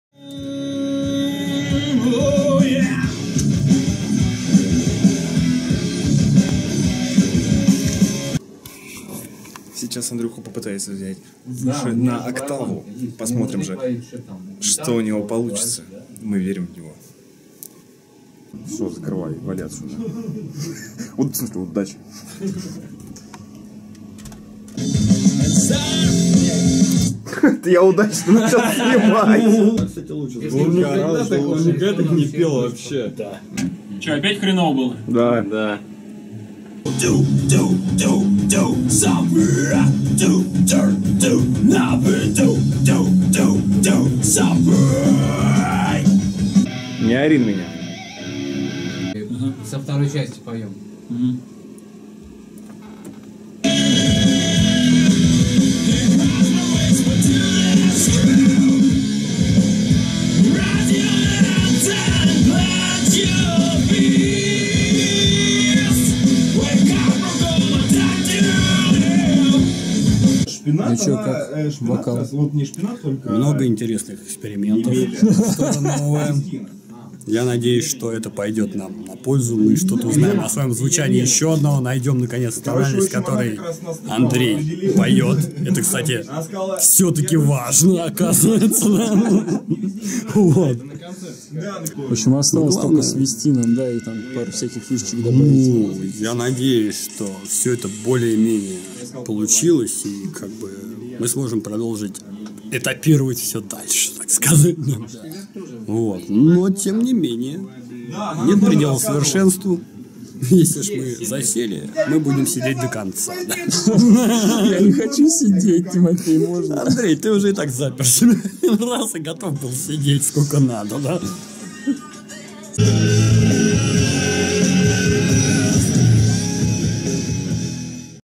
Sapless в студии - день последний. Вокал.
Этапирование, готовность сидеть до конца, эксперименты с октавами и немного вокала в последний день записи.